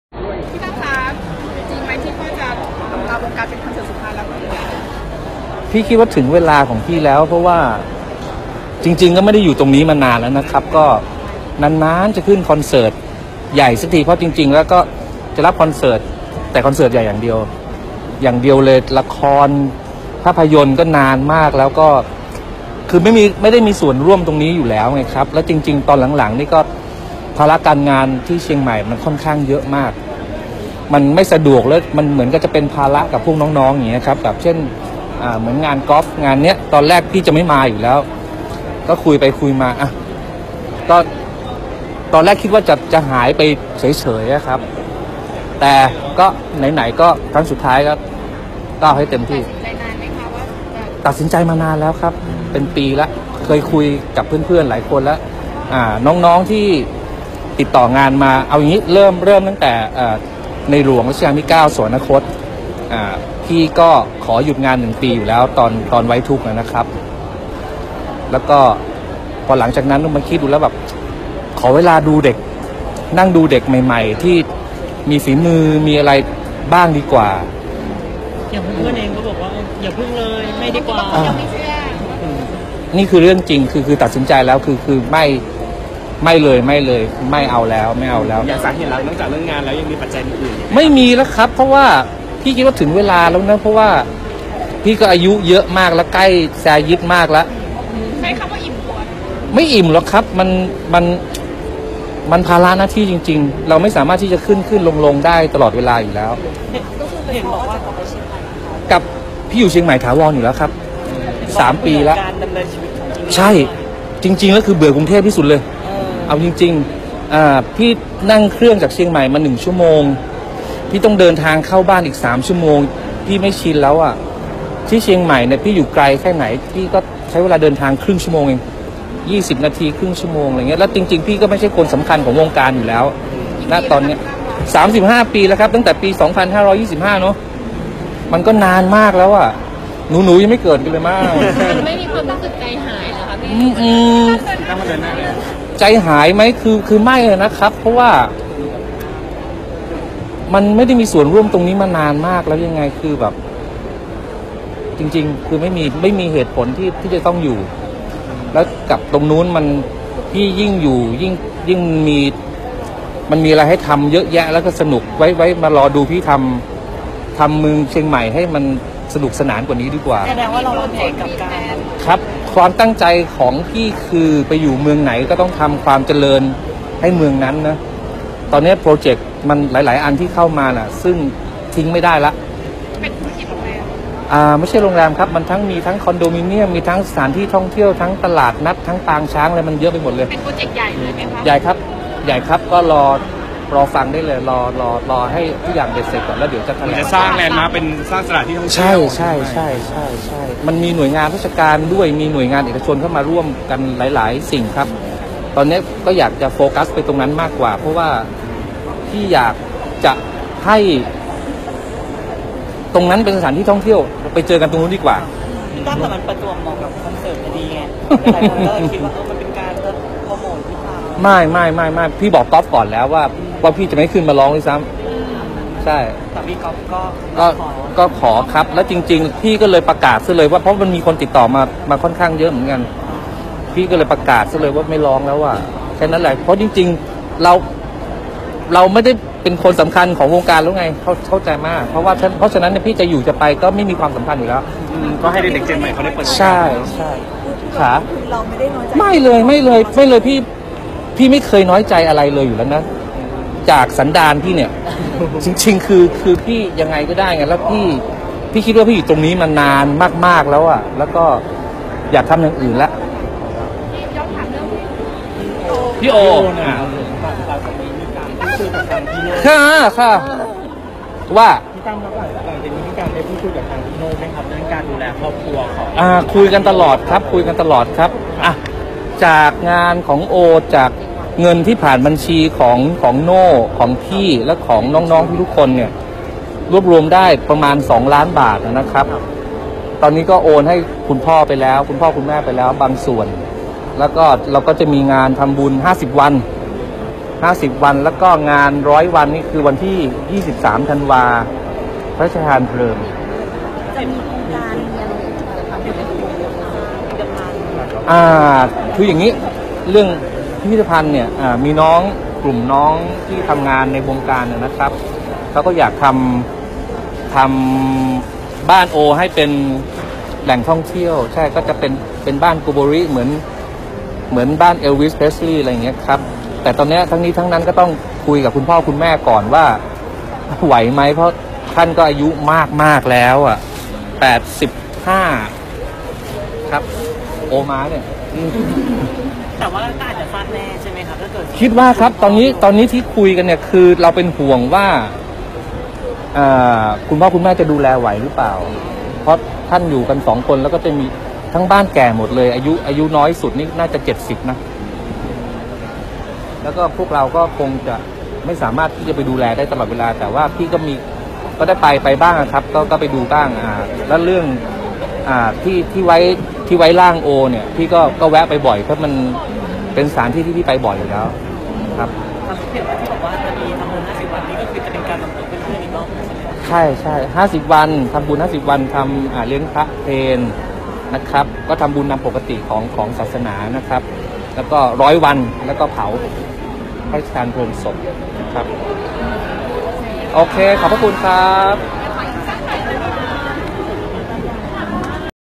ไม่ใช่คนสำคัญ! "ตั้ม" ลั่น 35 ปีในวงการถึงเวลาแซยิดแล้ว โอนเงิน 2 ล้านให้พ่อแม่ "โอ วรุฒ" บางส่วน
03/10/2018 ไม่ใช่คนสำคัญ! "ตั้ม" ลั่น 35 ปีในวงการถึงเวลาแซยิดแล้ว โอนเงิน 2 ล้านให้พ่อแม่ "โอ วรุฒ" บางส่วน
"ตั้ม สมประสงค์" ถึงเวลาอำลาวงการ ยันไม่ได้น้อยใจแค่ออกมาจากสันดานมากกว่า บอกเบื่อกรุงเทพฯ ขอรอดูฝีมือเด็กรุ่นใหม่ๆ ออกตัวไม่ได้เป็นคนสำคัญอะไร เปิดยอดเงินทำบุญงานศพ "โอ วรุฒ" ร่วม 2 ล้านโอนให้ครอบครัวแล้วบางส่วน เล็งเปิดบ้านเป็นแหล่งท่องเที่ยวเหมือน "เอลวิส เพรสลีย์" แต่ห่วงพ่อแม่ดูแลไหวหรือเปล่า